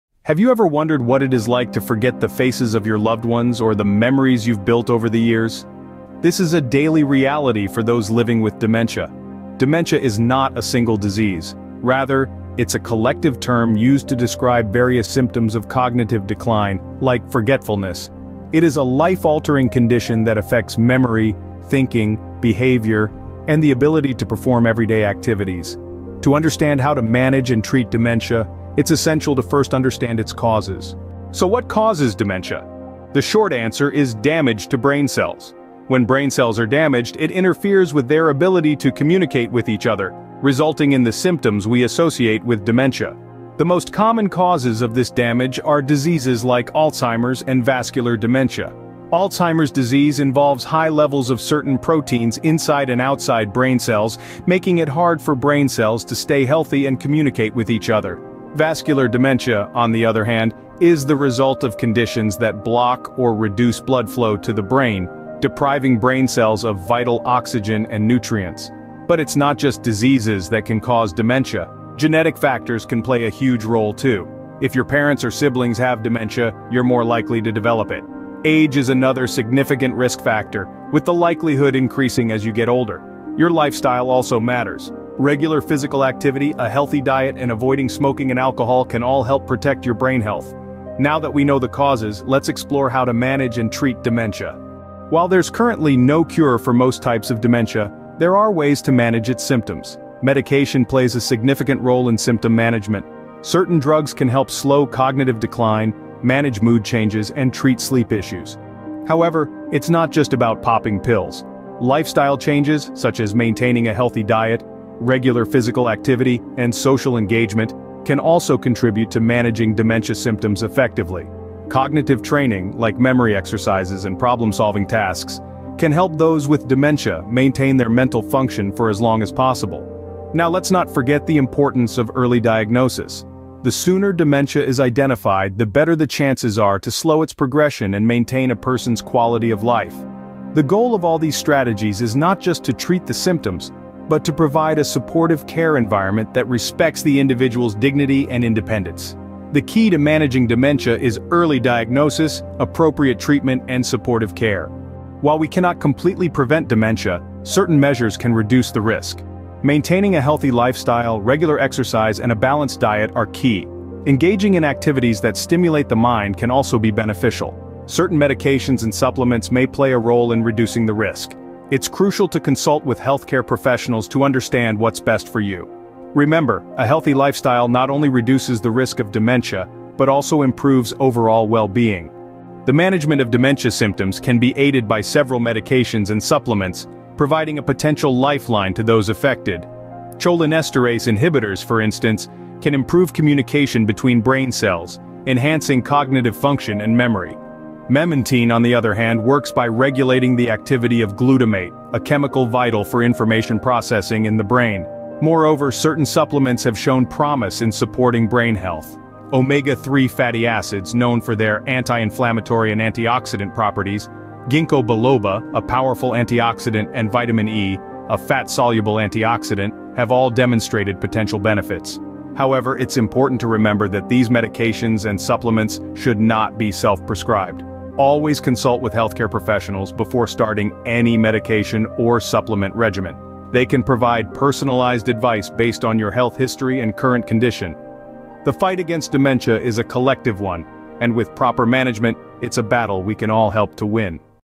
Dementia Decoded: Causes, Management, and Prevention
Welcome to our YouTube video on "Dementia Decoded: Causes, Management, and Prevention." In this comprehensive guide, we delve into the intricate world of dementia, exploring its multifaceted causes, effective management strategies, and practical tips for prevention. Join us as we unravel the mysteries behind this complex condition, shedding light on the biological, environmental, and lifestyle factors that contribute to its development. From understanding the role of genetics to exploring the impact of diet and exercise, we provide valuable insights to empower you in navigating the challenges associated with dementia. Whether you're seeking answers for yourself, a loved one, or simply aiming to broaden your knowledge, this video is designed to equip you with the information you need to make informed decisions and take proactive steps towards brain health. Together, let's continue our journey towards greater understanding and empowerment.

OUTLINE:
00:00:00  "Understanding Dementia"
00:00:31  "Causes of Dementia"
00:01:34  "Managing and Treating Dementia"
00:02:34  "Preventing Dementia"
00:03:02  "Medications and Supplements for Dementia"